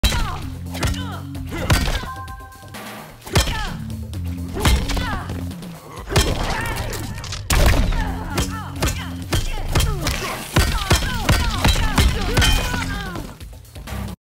26% Jax corner combo (read desc)
Alright, to be honest, this wasn’t something I planned out. I was messing around and I did this on accident so I decided to clip it. Basically this is the combo. B2 KB,1,1,1,D1,DB1.